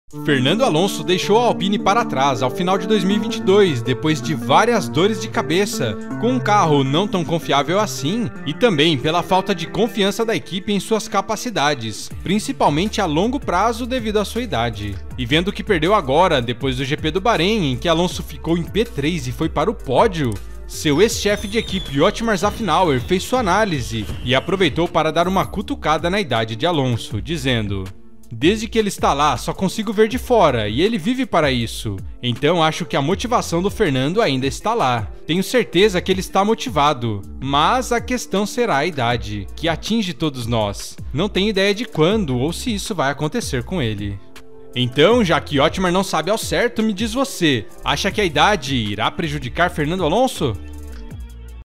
CHEFE DA ALPINE ANALISA IDADE E DESEMPENHO DE ALONSO NA ASTON MARTIN | FÓRMULA 1 | GP EM CASA
Fernando Alonso deixou a Alpine para trás ao final de 2022 depois de várias dores de cabeça com um carro não tão confiável e também, pela falta de confiança da equipe em suas capacidades, principalmente à longo prazo, devido à sua idade.